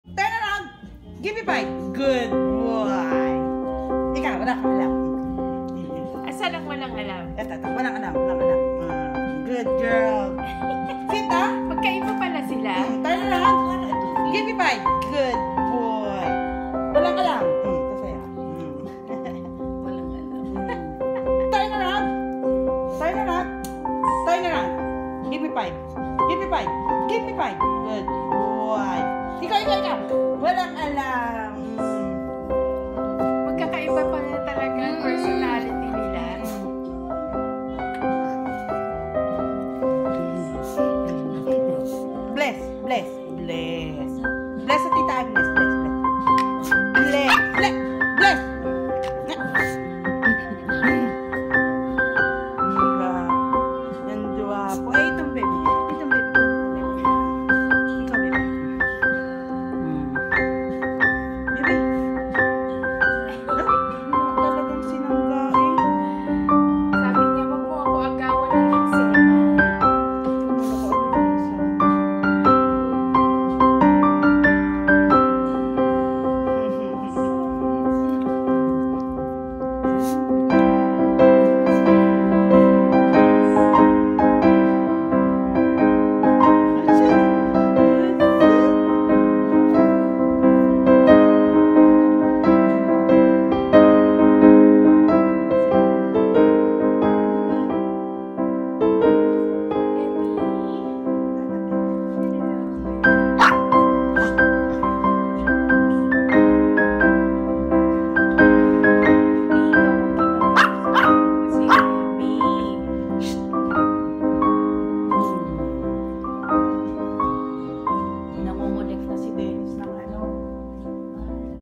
Filipina Nurse life in Australia: Personality Traits of Chihuahua Panda & Emie.
ML  PLEASE watch live now